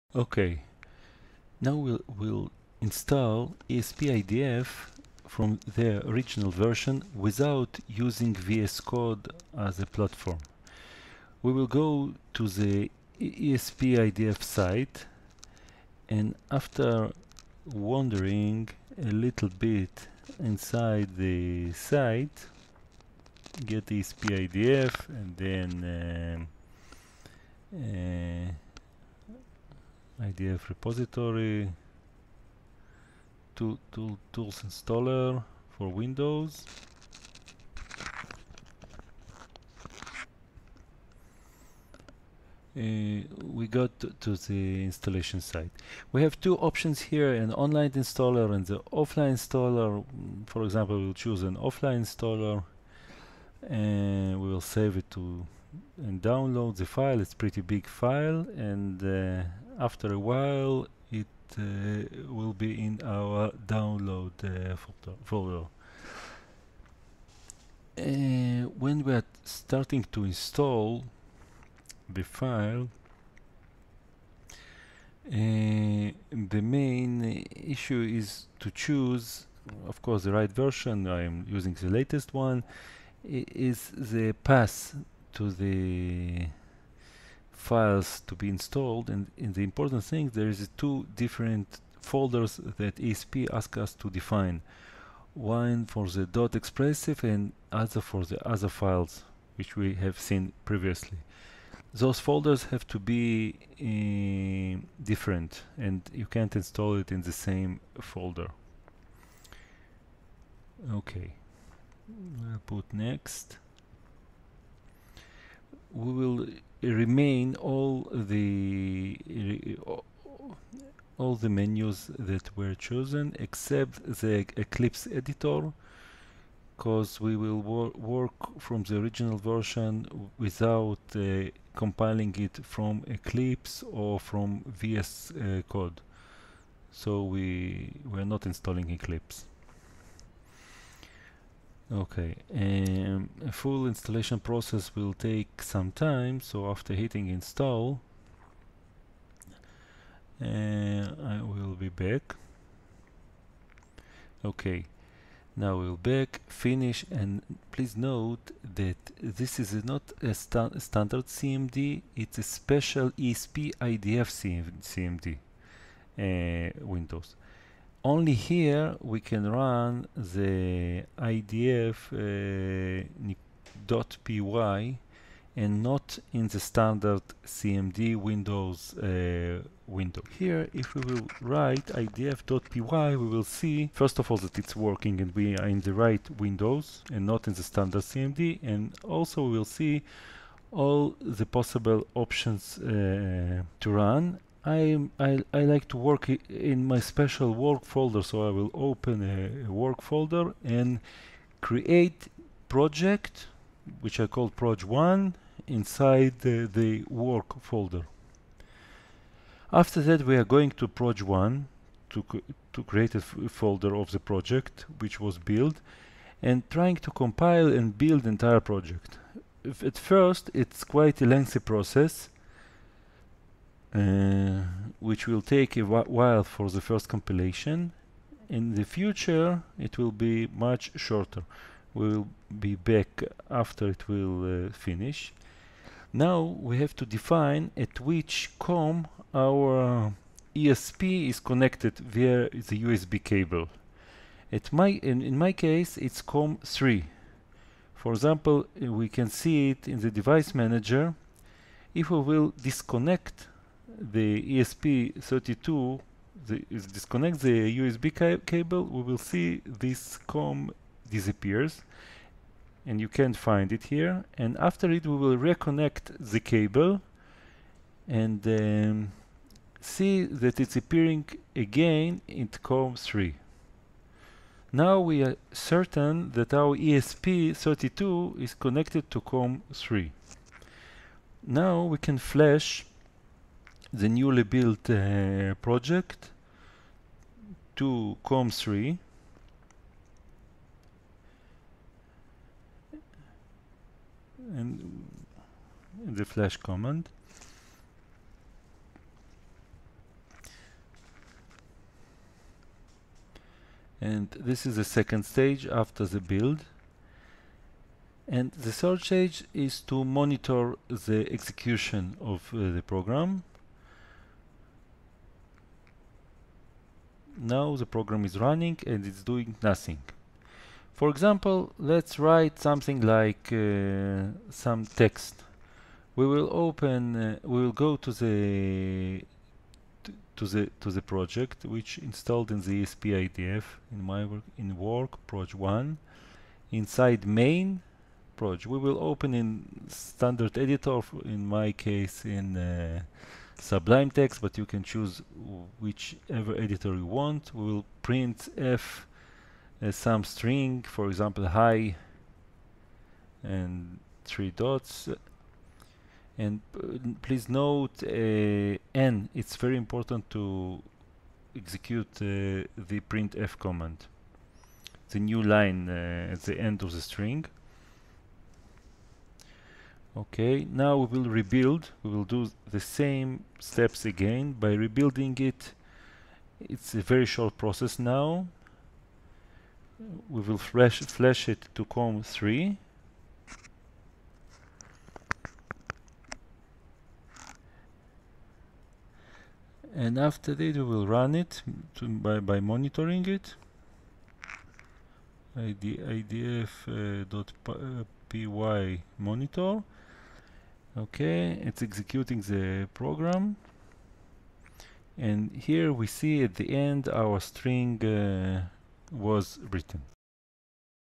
Original ESP IDF installation for esp32
Install ESP IDF for esp32 from the original site and run "Hello world" by using arbitrary editor.